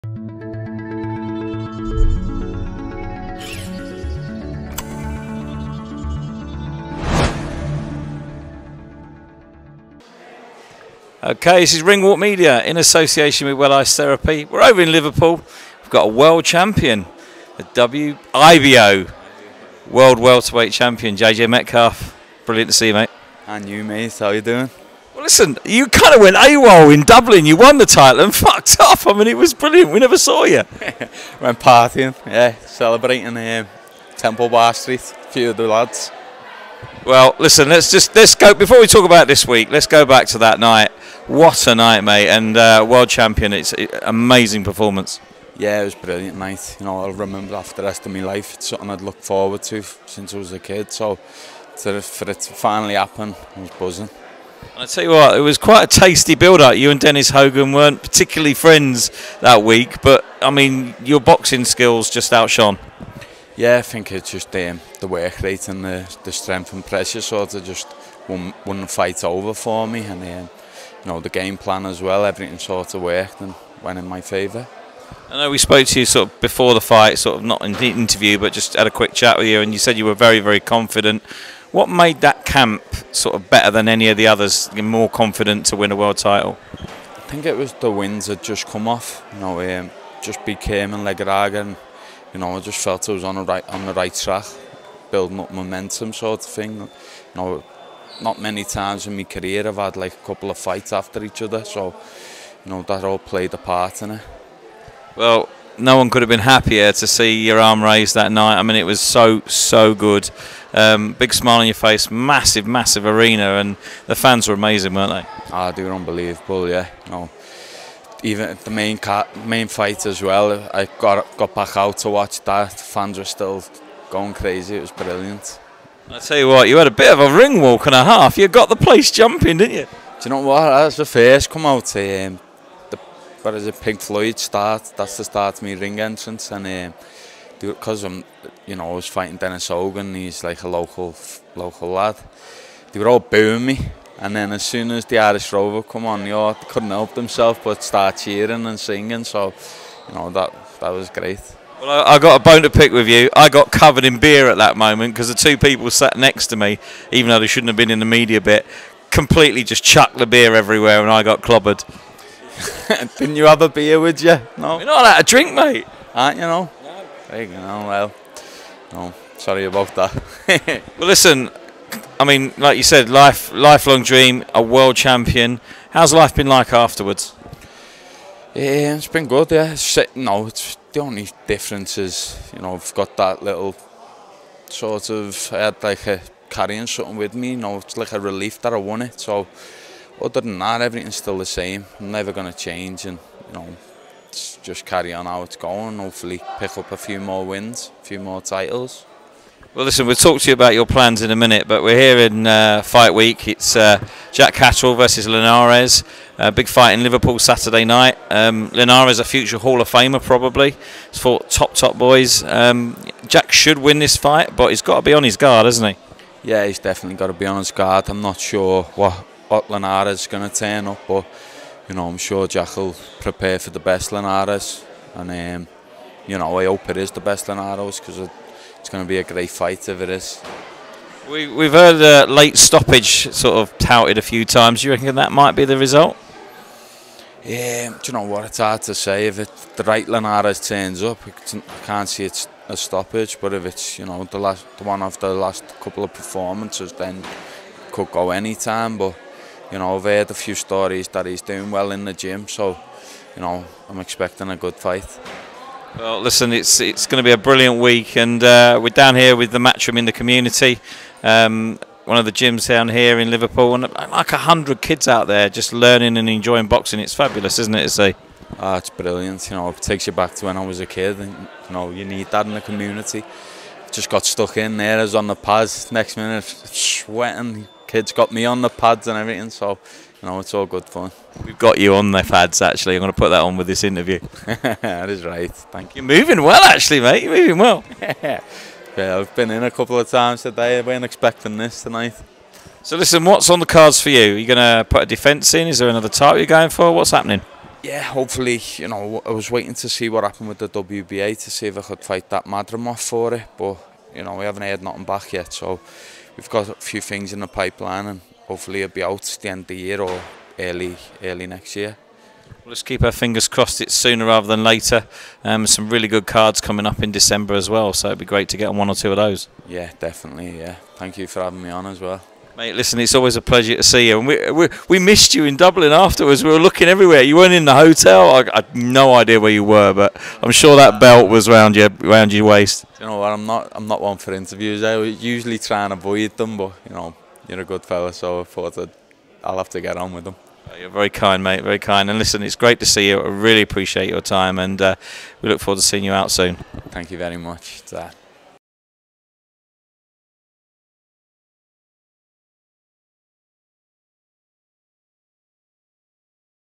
IBO WORLD WELTERWEIGHT CHAMPION “KID SHAMROCK” aka James Metcalf Interview.
A lifelong dream was realised in May in Dublin when James Metcalf became a world Champion. Always relaxed and such a nice guy, we speak about the fight and also get his thoughts on the Jack Catterall v Linares fight in Liverpool.